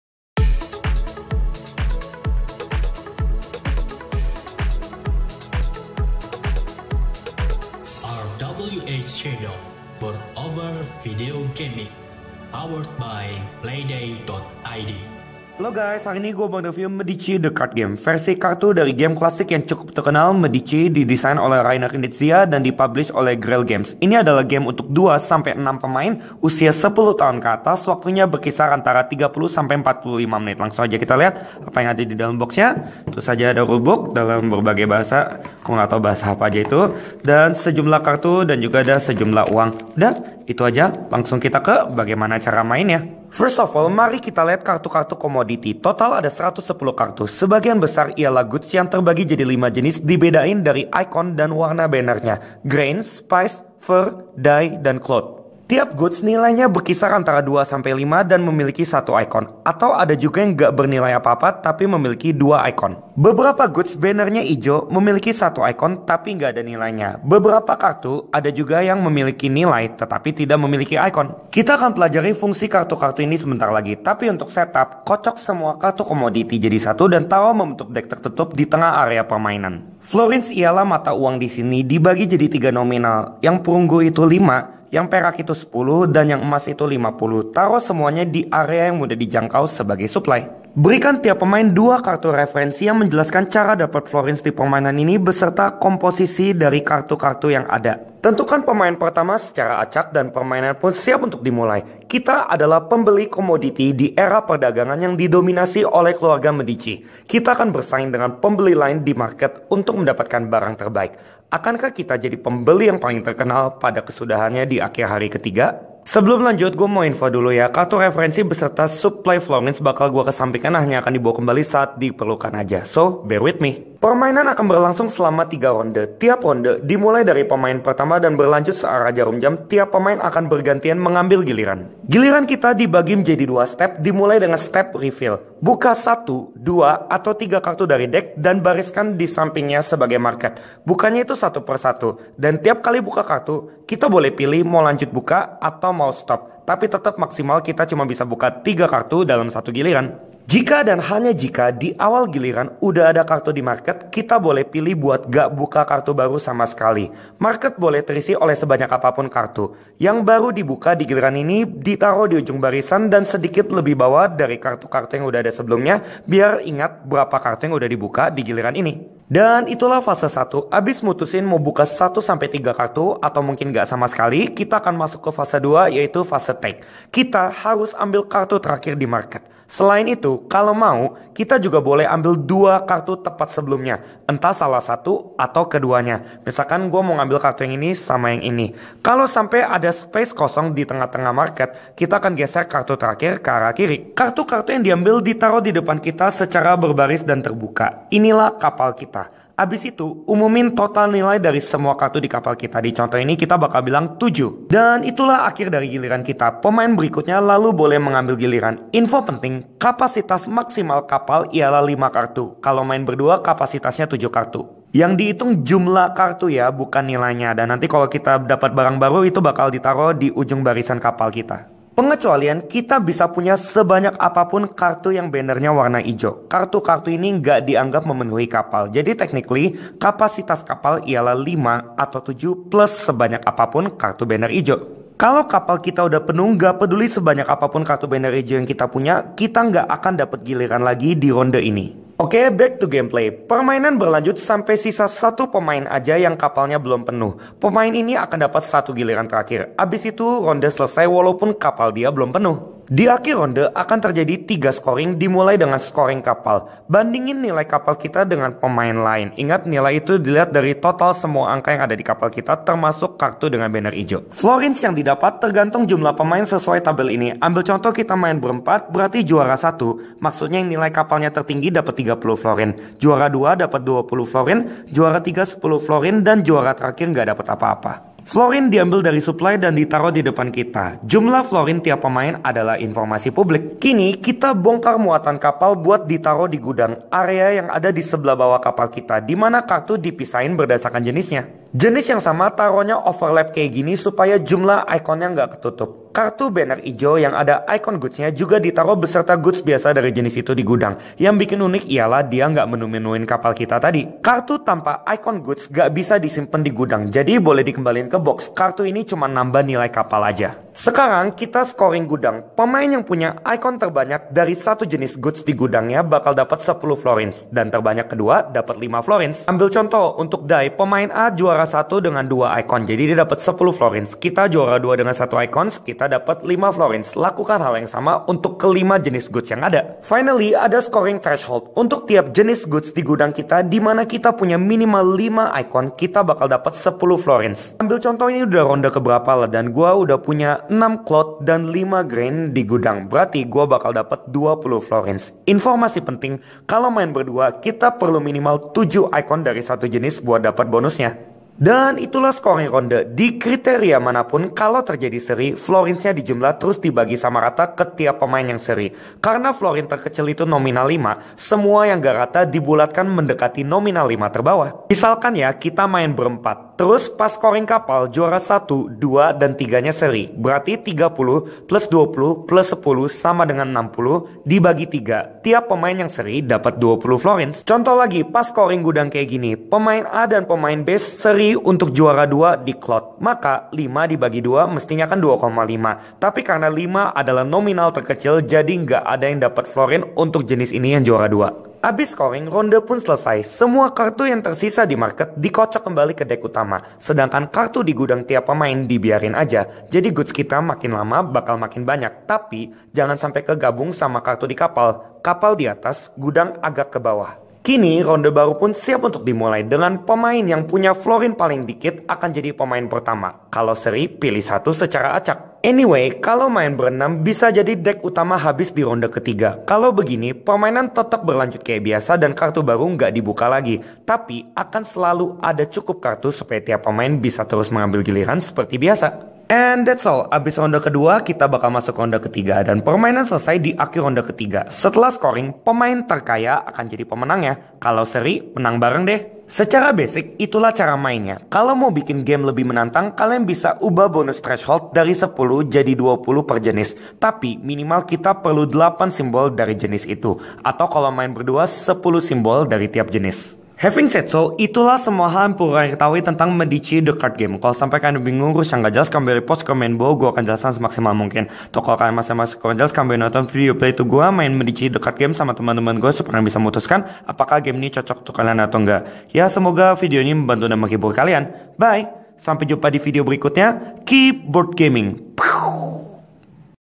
Tutorial Main Board Game - Medici The Card Game
Versi sederhana dari Auction Game klasik yang terkenal, Medici (Well, di ini bukan Auction Game tapi).. Tanpa mengubah mekanismenya secara signifikan, kini Reiner Knizia merangkumnya dalam box yang lebih kecil.. Jadi apakah trip kita selama 3 hari di Italia? Akankah kita jadi saudagar yang terkenal dengan membeli barang-barang yang terbaik?

Mau membeli Board Game dari Toko yang terpercaya? Yuk cek link berikut!